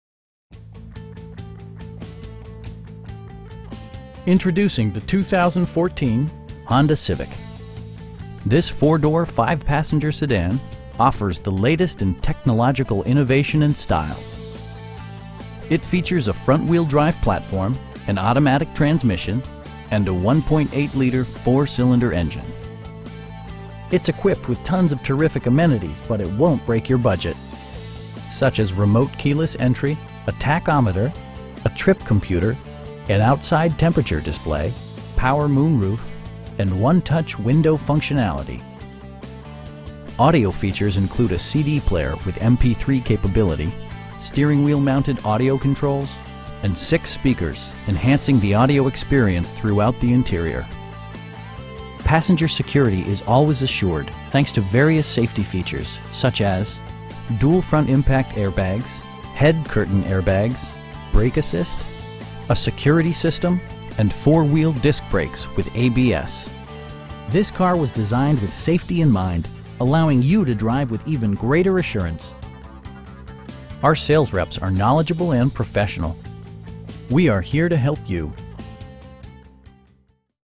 2014 Honda Civic EX in Greensboro, NC 27407
3633 W. Wendover Ave

Introducing the 2014 Honda Civic This 4 door, 5 passenger sedan offers the latest in technological innovation and style. It features a front-wheel-drive platform, an automatic transmission, and a 1.8 liter 4 cylinder engine. It's equipped with tons of terrific amenities, but it won't break your budget. Such as remote keyless entry, a tachometer, a trip computer, an outside temperature display, power moon roof, power windows, cruise control, and 1-touch window functionality. Audio features include a CD player with MP3 capability, steering wheel mounted audio controls, and 6 speakers, enhancing the audio experience throughout the interior. Passenger security is always assured thanks to various safety features, such as: dual front impact airbags, head curtain airbags, traction control, brake assist, a security system, and 4 wheel disc brakes with ABS. This car was designed with safety in mind, allowing you to drive with even greater assurance. Our sales reps are knowledgeable and professional. We'd be happy to answer any questions that you may have. We are here to help you.